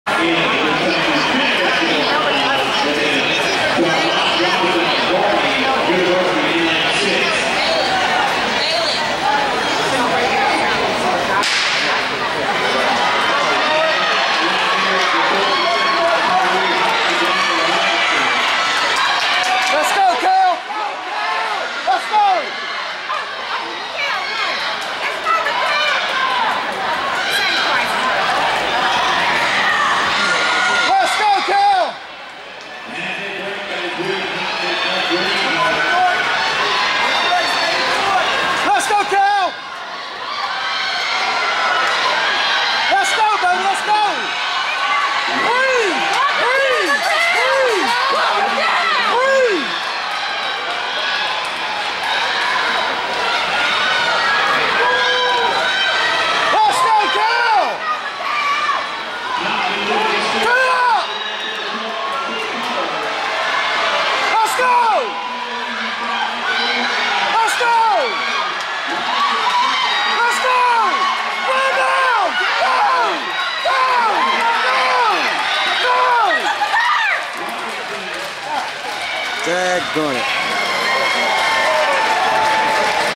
Canoy Tyree Finishes 3rd in the 500 at the 2017 NCHSAA 4A Indoor State Championships!
Canoy Tyree runs the 500m run for Mallard Creek High School at the 2017 NCHSAA Indoor State Championships on February 11, 2017 and finished third with a time of 1:05.80.  Prior to this race Canoy split a 1:58.13 on his leg of the 4 X 800 relay in which his team finished in 2nd place. Unfortunately, Canoy cramped up on his side just before the start of the race. He ran through the cramp to get the third place finish. The meet was held at JDL FastTrack in Winston-Salem, NC.